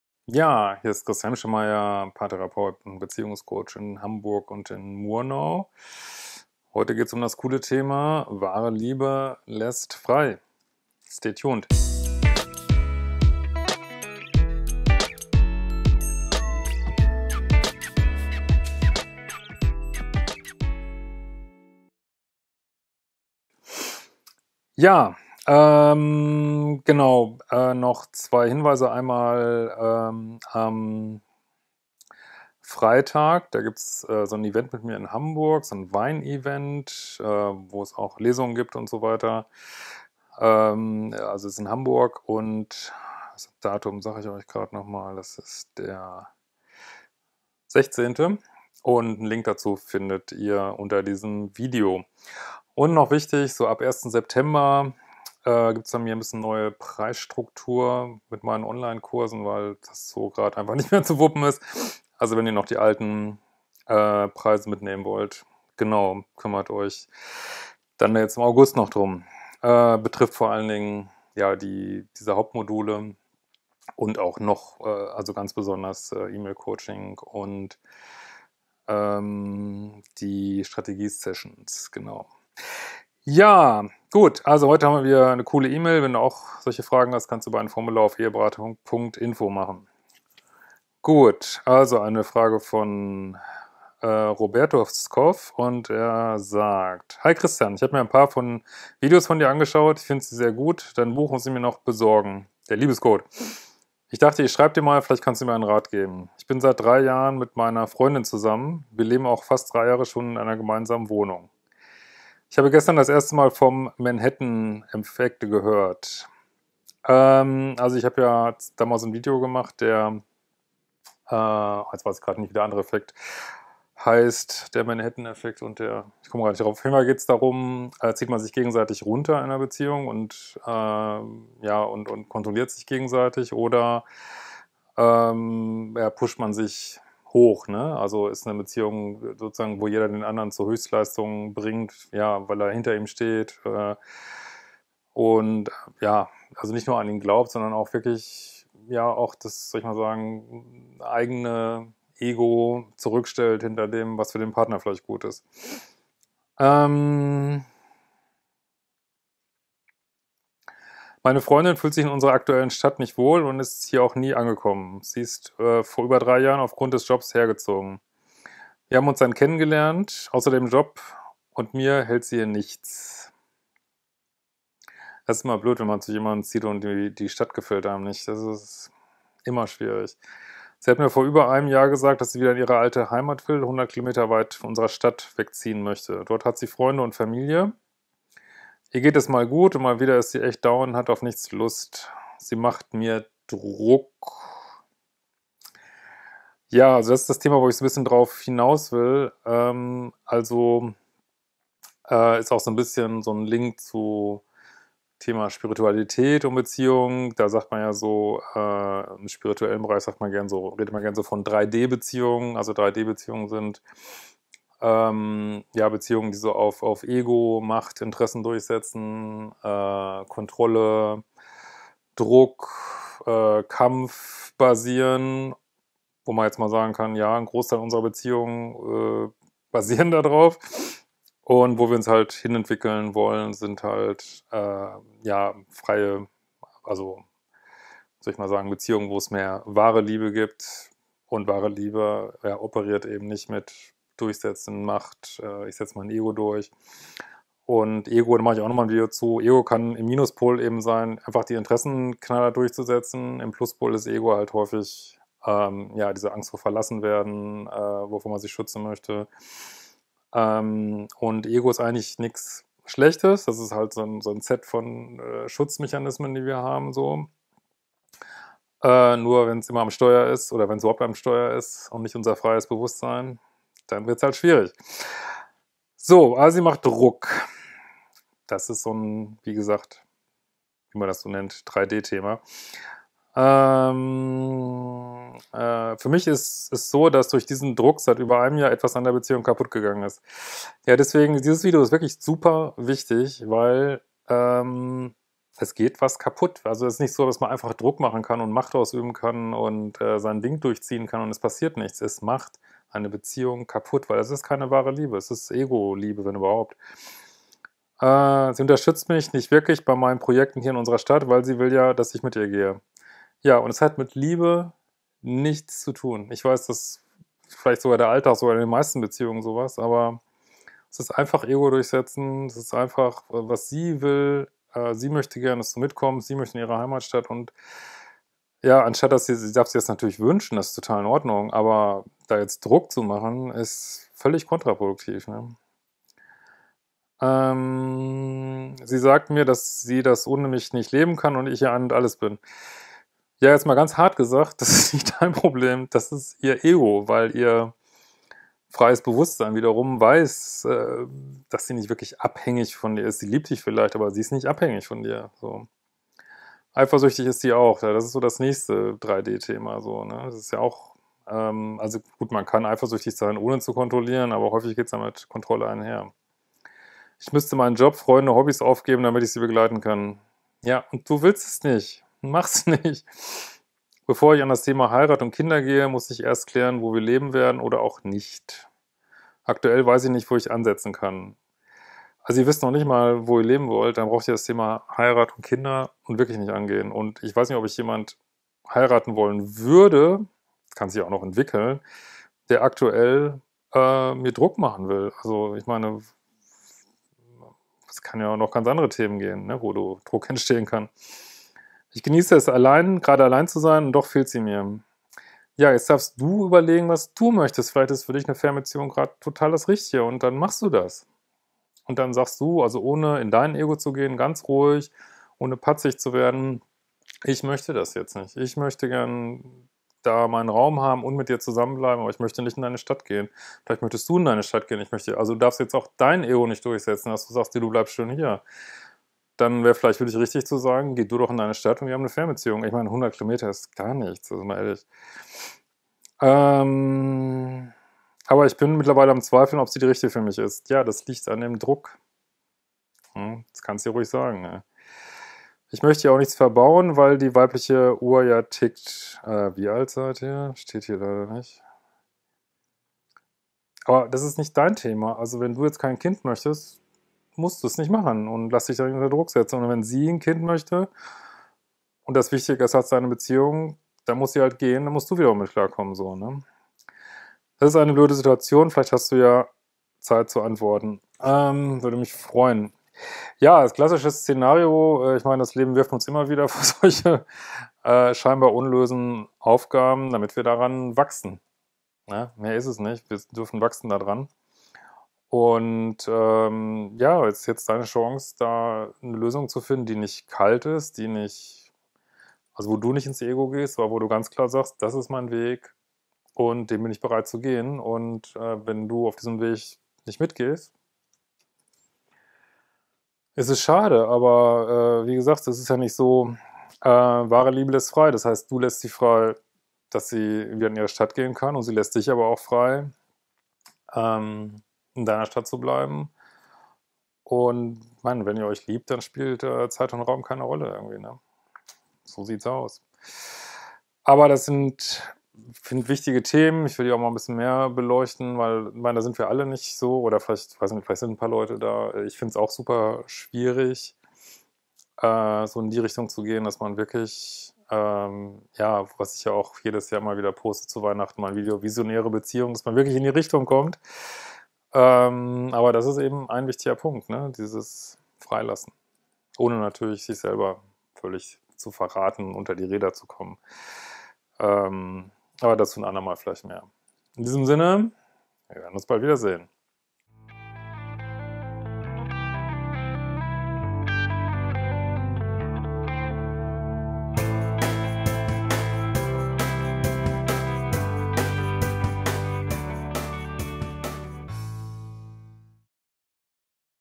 WAHRE LIEBE LÄSST FREI
Eine wunderschöne Zuschauerfrage, scheinbar nur zum Thema "wo wohnen wir". Tatsächlich offenbart sich dort aber das Dilemma vieler heutiger Beziehungen.